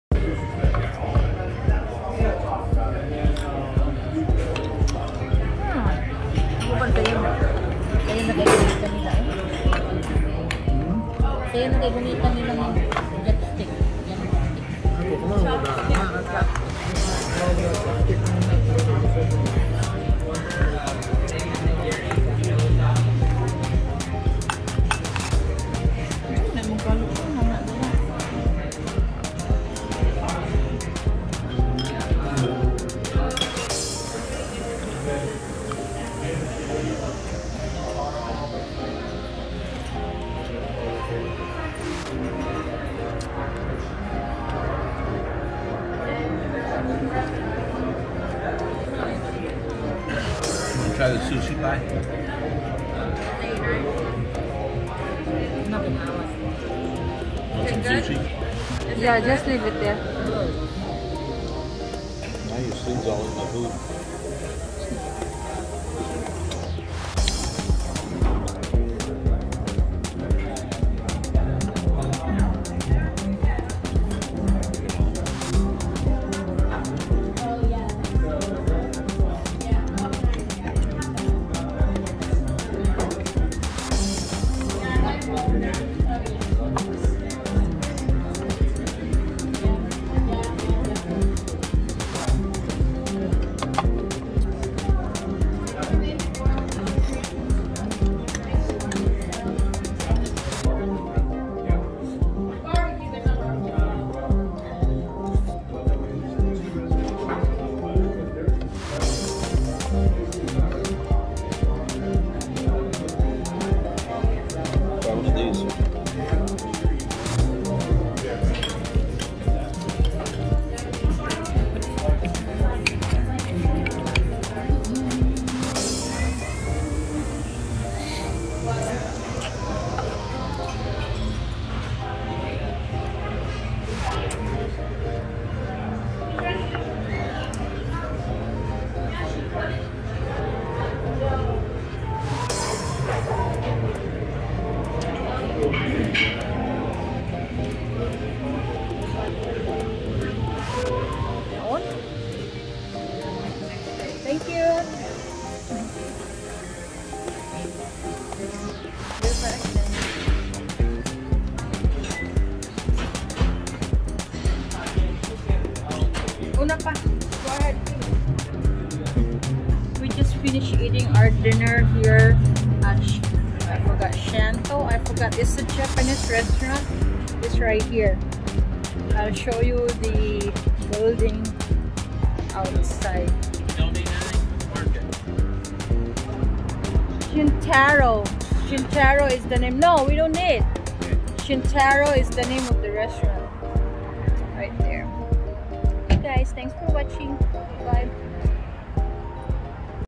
Shintaro Japanese Restaurant in Hollywood-LA, California |How to lose weight without diet?(Valentus)
Shintaro Japanese Restaurant, at this strip-mall spot, diners eat Japanese fare at wooden tables & chairs or at the sushi bar.
Dine-in · Takeout · Delivery
Address: